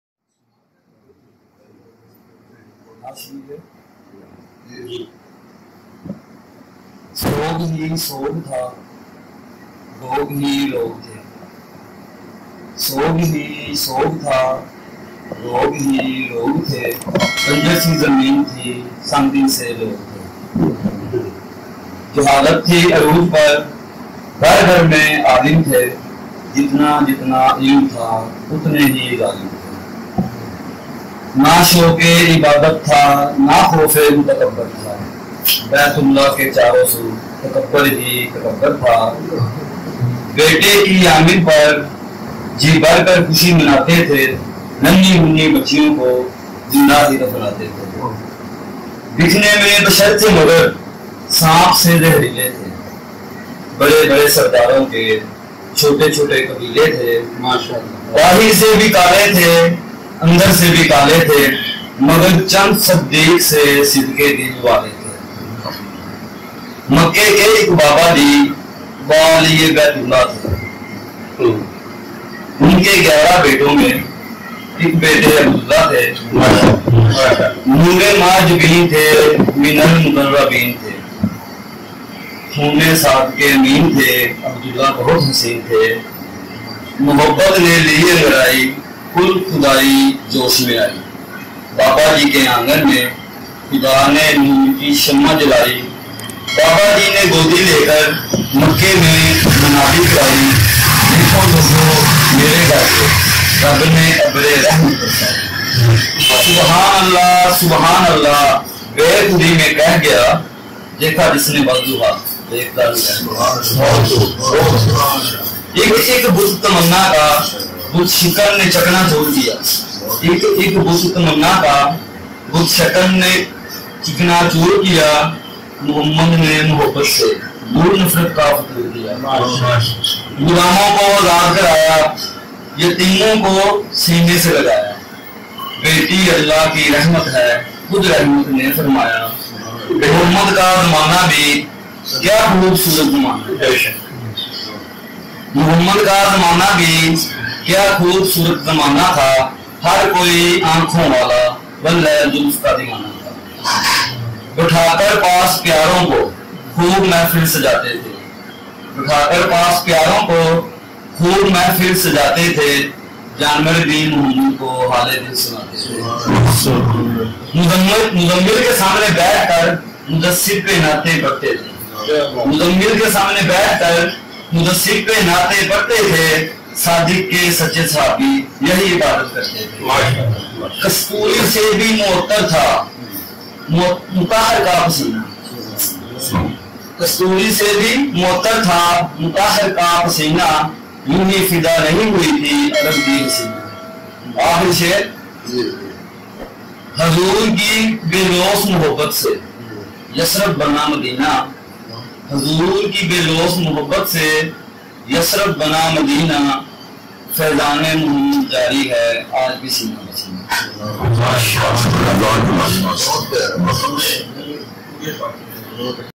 Best Poem | by | Dr Sami | Urdu Kalam | JMS Technology | Bazme Adam | T.M.Khan | Urdu Poetry
سوگ ہی سوگ تھا، لوگ ہی لوگ تھے
بنجر سی زمین تھی ، سنگ دل سے لوگ تھے
جہالت تھی عروج پر ، گھر گھرمیں عالم تھے
جتنا جتنا علم تھا ، اُتنے ہی عالم تھے
نہ شوقِ عبادت تھا ، نہ خوفِ تکبر تھا
بیتُ اللہ کے چاروسو ، تکبر ہی تکبر  تھا
بیٹے کی آمد پر ، جی بھر کر خوشی مناتے تھے
ننّی منّی بچیوں کو ، زندہ ہی دفناتے تھے